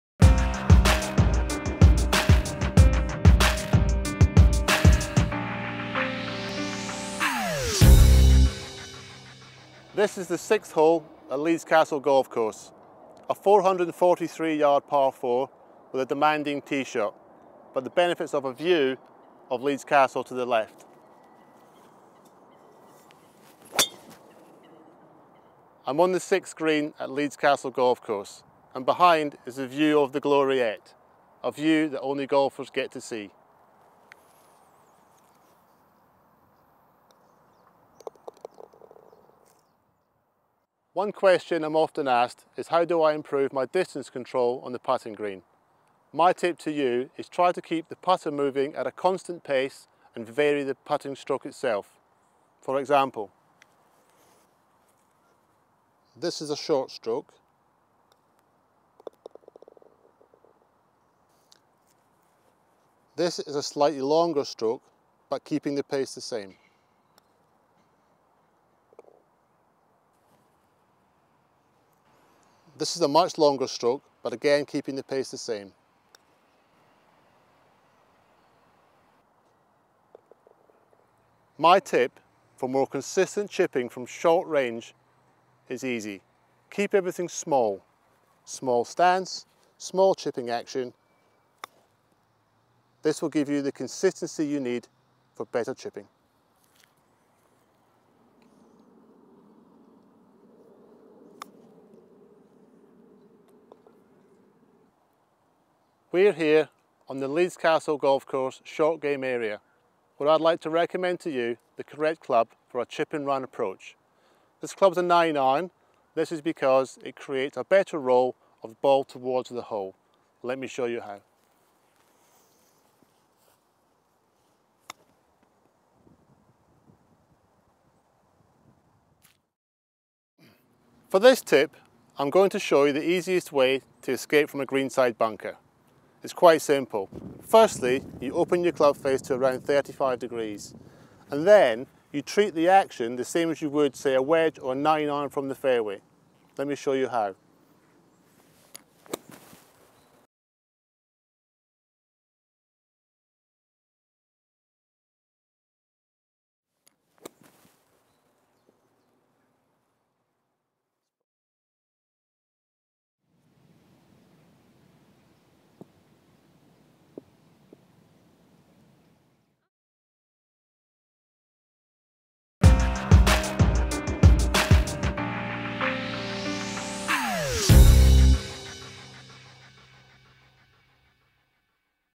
Leeds Castle Golf Tips
Need help with your golfing techniques? Our PGA Golf Pro Steve is on hand with these handy tips...